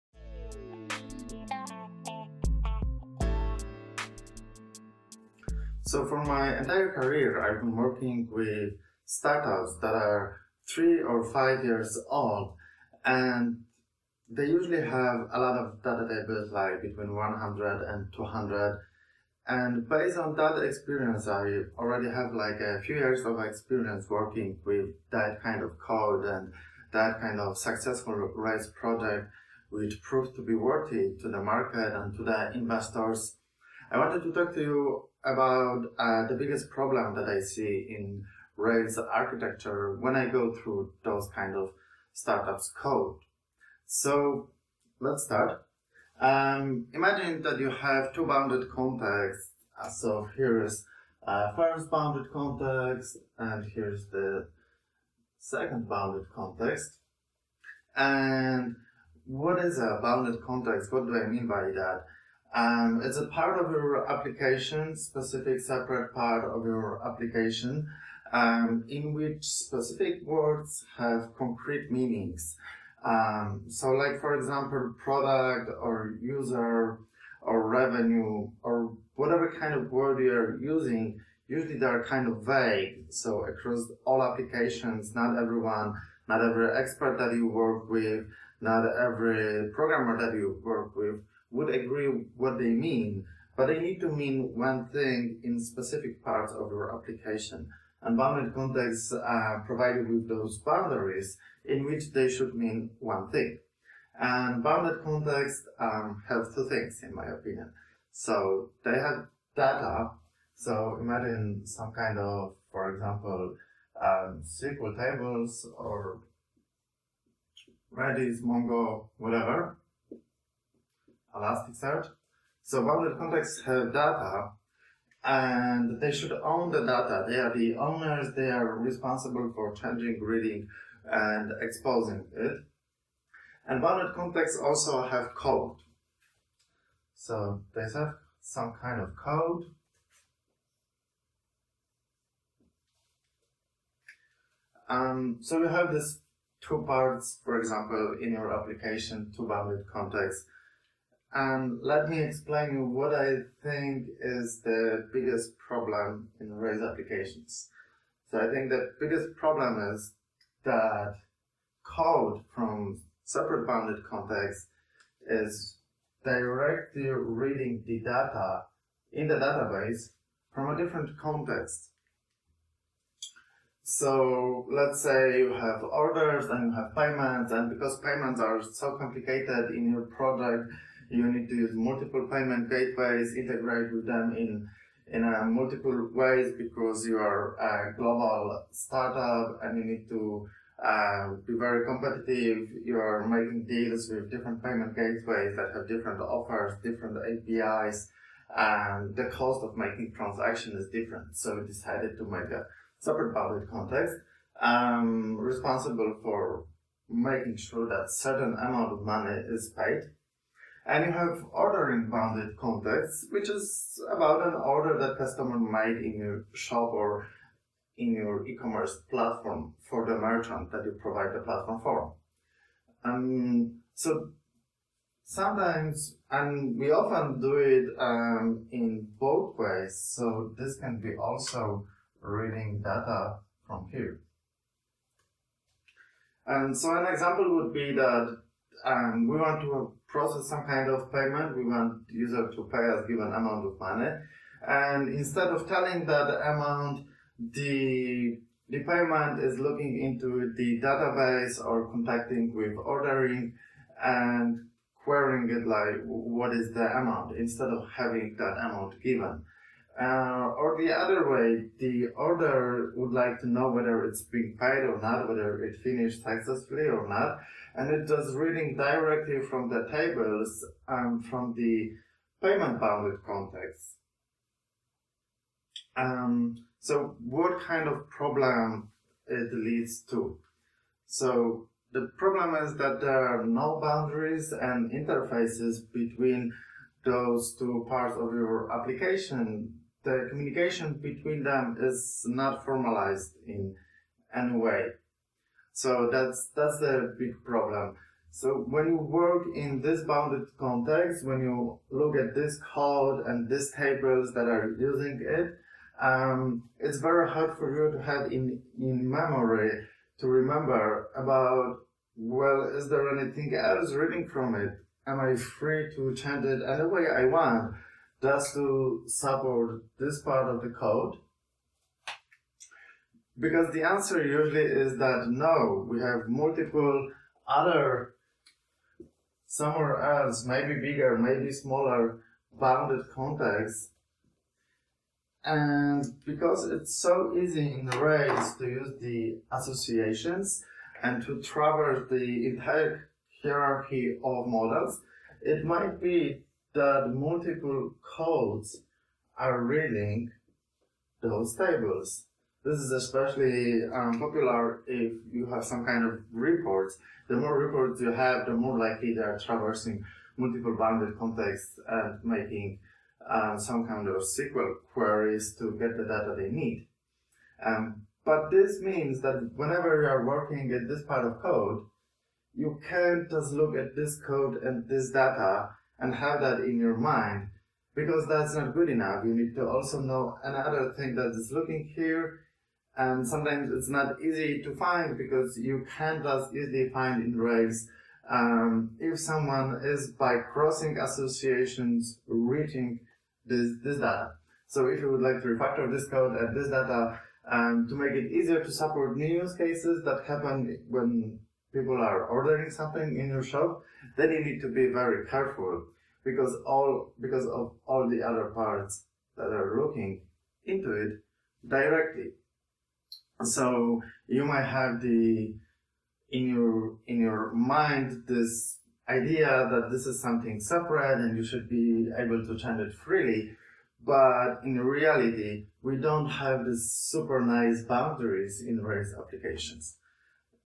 Isolate bounded contexts
Isolate your bounded contexts and avoid the biggest architectural problem with Rails apps. Find books and videos which help to achieve such goal